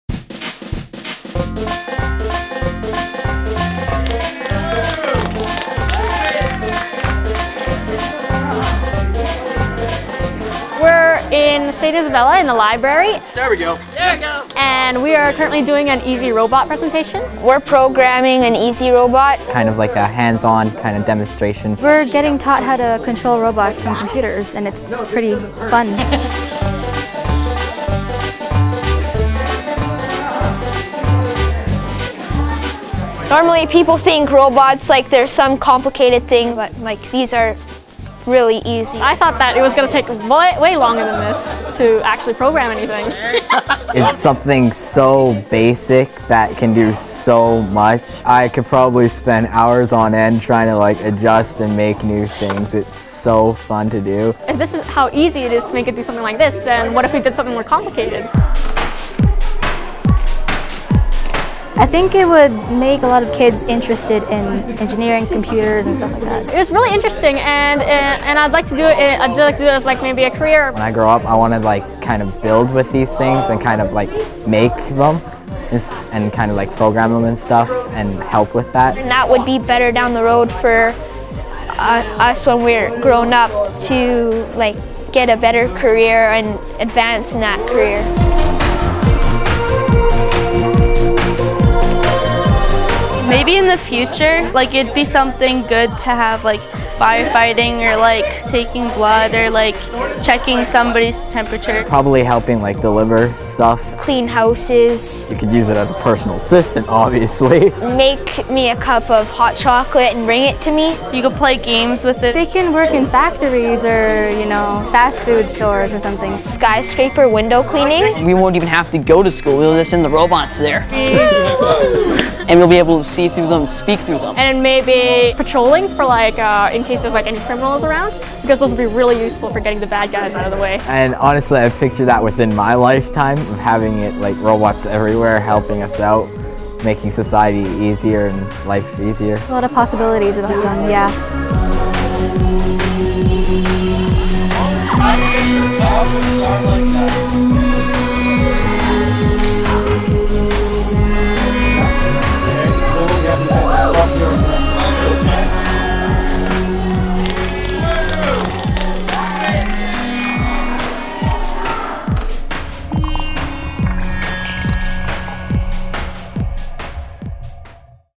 House of Robots: Robot Education
Kids love robots. Plain and simple. EZ-Robot makes robots fun and exciting for kids, and I'm doing the same thing with my new book, House of Robots.

We are in City’s Villa in the library.  There we go.  Yes.  And we are currently doing an easy robot presentation.  We are programming an easy robot.  Kind of like that hands on.  Kind of demonstration.  We’re  getting  taught how to control robots through computers and it’s pretty fun.

Normally people think robots some  complicated thing but like these are really easy.  I thought that it was gonna take way long than this to actually program anything.

It’s something so basic than can do so much.  I can probably spend  hours on it trying to adjust and make new things.  It’s so fun to do.  And this is how easy it is to make a design like this than what if we do something more complicated.

I think it would make a lot of kids interested in engineering, computers and stuff like that.  It;s really interesting and I’d like to do maybe a career.

When I grow up I wanted to build these things and kinda like of make them and kinda like program with these stuff and help with that

That would be better down the road for us when we are grown up to like get a better career and advance in that career.

Maybe in the future like you”d be something good to have like firefighting or like taking blood or like checking somebody’s temperature.

Probably helping like the weather stuff.

Clean house.  You can use it as a personal assistant.  Obviously.  Make me a cup of hot chocolate and bring it to me.  You can play games with it.  They can work in factories or you know fast food stores or something.

Skyscraper.  Window cleaning.  We wont even have to go to school.  We listen to robots there.  And we be able to see through them and speak with them.  And maybe patrolling like in cases when criminals around because that would really be useful in getting the bad guys out of the way.  And honestly I’d stick to that within my lifetime and having it robots everywhere, helping us out. Making society easier and life easier.

It’s not a possiblity.  It’s a         yeah